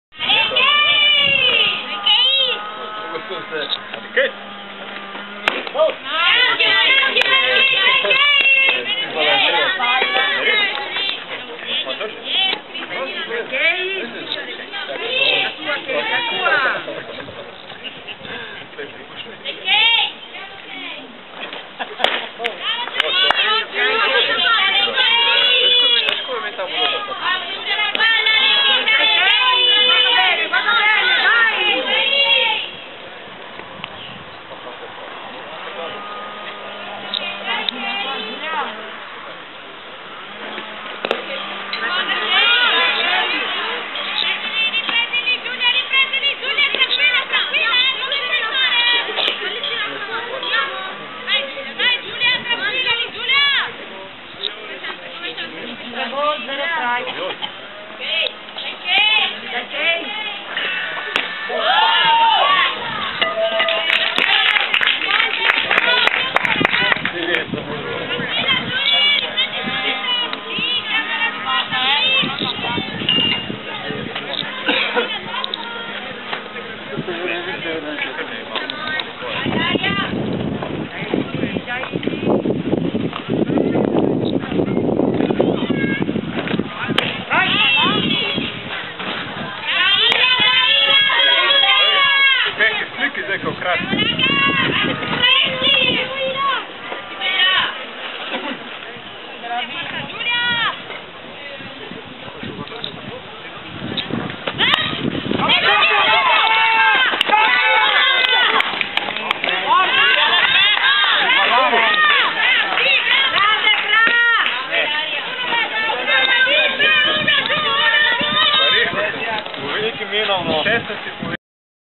Castionese / Jr. Alpina - Rovigo 16 aprile 2011 parte 3
Spezzone di gioco fra la franchigia Castionese / Jr. Alpina e Rovigo softball, partita valida per la serie B 2011 softball, prima giornata.